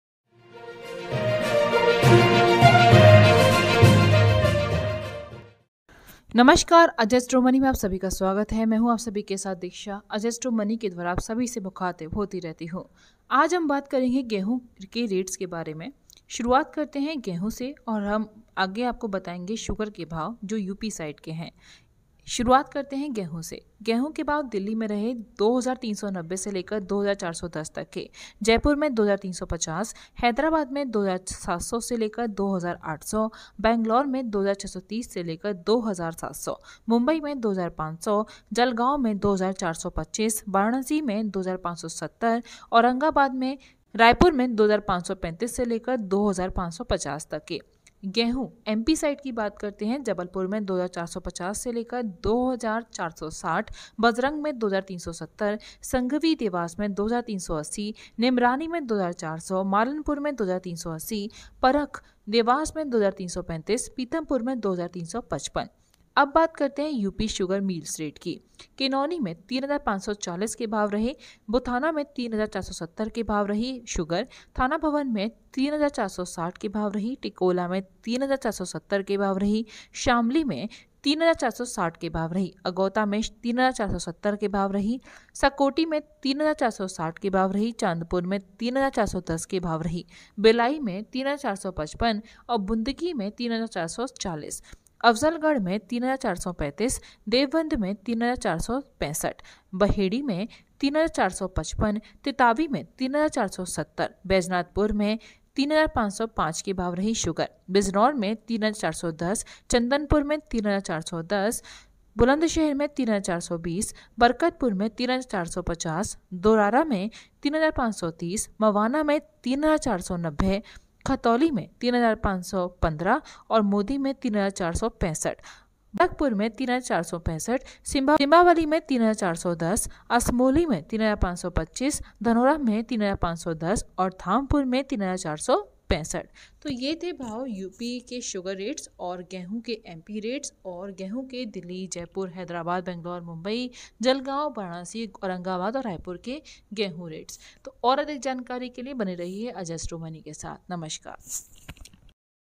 जाने गेहूं और शुगर रेट्स दीक्षा के साथ
60 टन गेहूं के निर्यात का अनुमान/गेहूं के अंतिम स्टॉक में 55% की बड़ी भारी गिरावट

Sugar Rates/ गुजरात महाराष्ट्र तमिलनाडु एवं मध्य प्रदेश की चीनी मिलों के आज के ताजा भाव

भारत में 12 रुपये लीटर मिलेगा कच्चा तेल अगर इस बिजनेस मैन की बात मान ली जाए देखे पूरी रिपोर्ट